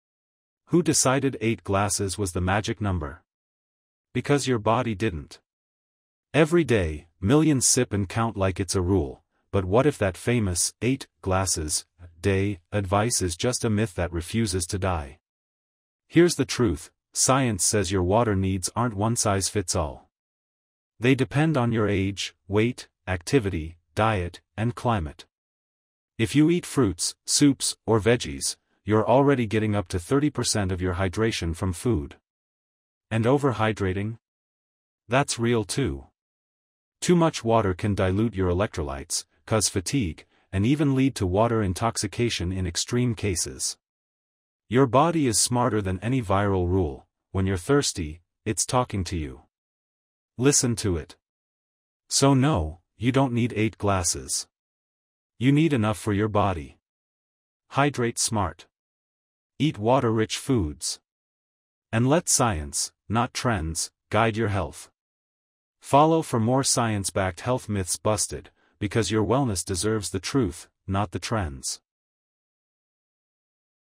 Do You Really Need 8 Glasses of Water a Day? (Myth Busted!)
Think drinking 8 glasses of water a day keeps you healthy? Think again. 🧠 This video busts one of the biggest health myths ever told — the “8 Glasses Rule.” Discover what science really says about how much water your body needs, the hidden dangers of overhydration, and how to stay truly hydrated the right way. 🌿